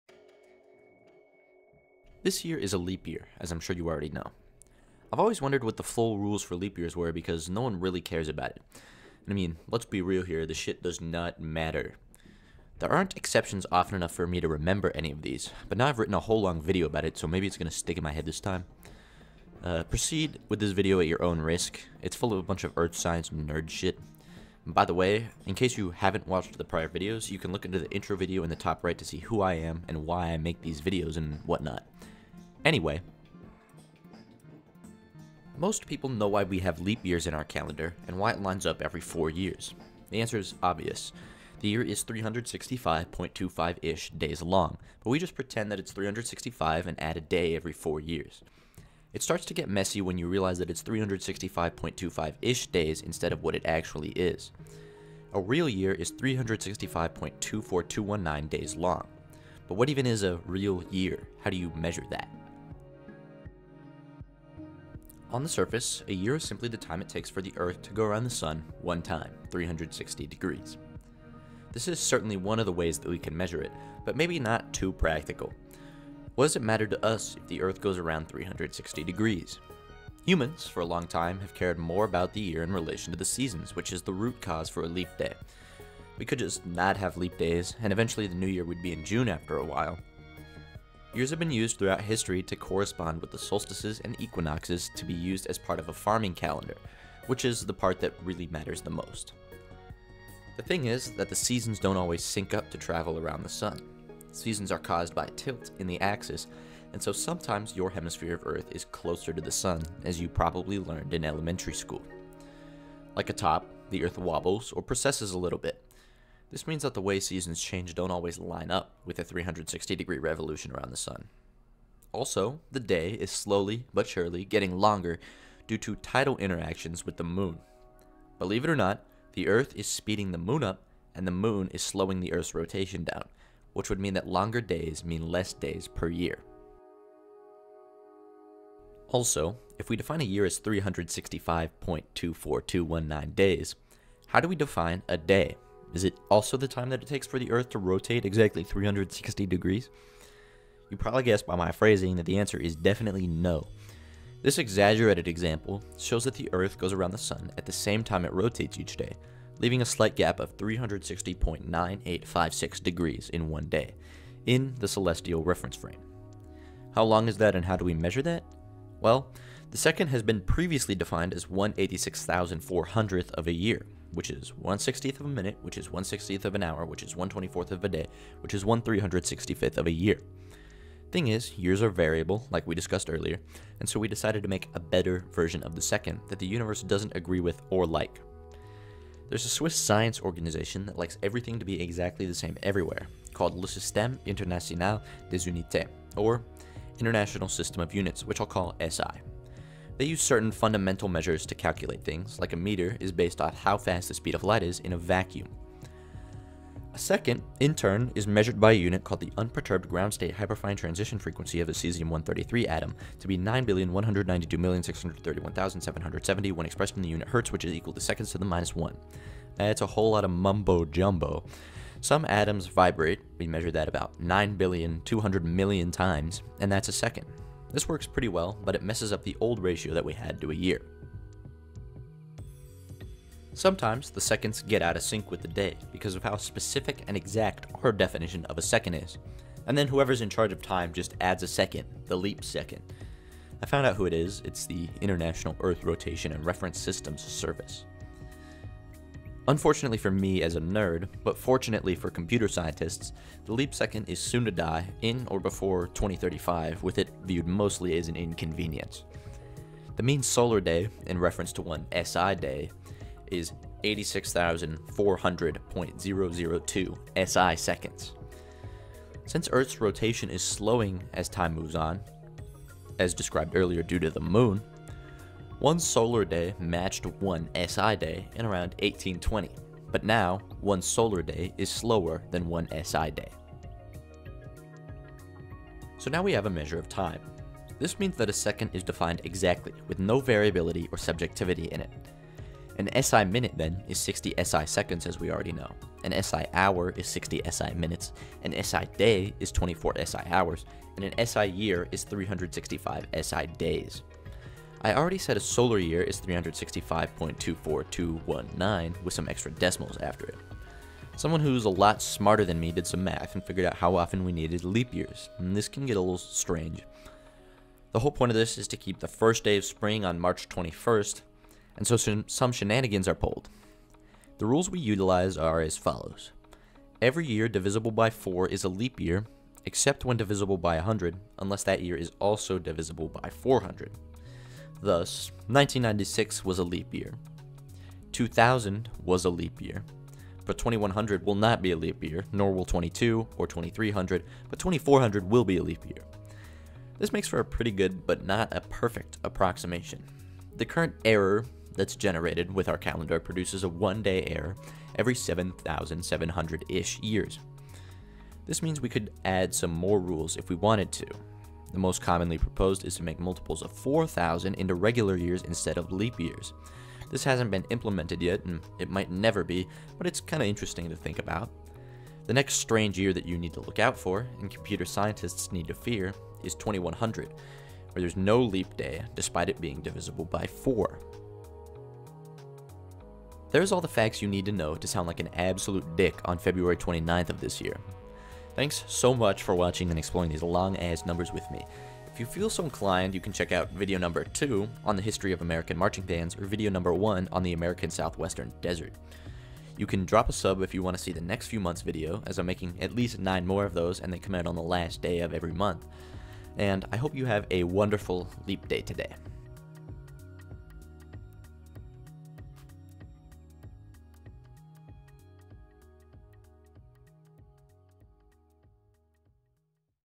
3 - Leap years are a Little Strange
Leap Years are always a little intriguing, as I’ve heard there are other rules about leap years besides every year divisible by 4. This February 29th of 2024 is a leap year, and so it seemed only appropriate to cover the oddities of the SI second, Caesium atoms, oscillations, top precession, and all the other strange Earth Science phenomena that affect how we measure time and count days.

CHAPTERS HERE:
00:00 - Intro
00:34 - Chapter 1: The Basic Question
01:03 - Chapter 2: What is a Year?
02:18 - Chapter 3: Time is made up; the Universe Hates us.
03:52 - Chapter 4: The Legendary Leap Second
04:45 - Chapter 5: The Rules of Leap Years
06:22 - Outro

Journal Sources:
Meeus, Jean, and Denis Savoie. “The History of the Tropical Year.” Journal of the British Astronomical Association, vol. 102, no. 1, Feb. 1992.
Allen, Clabon Walter, and Arthur N. Cox. Allen’s Astrophysical Quantities. AIP Press, 1999.

All Animation is done by hand with references to different generic images.

Music Sources: Thanks again to Danny at C418 for his generous use policy as outlined here:

Video 3:
Due on 02/28/24 2359 CST
Scheduled for 02/28/24 2359 CST
Uploaded to YouTube at 0724 CST 02/28/24
oops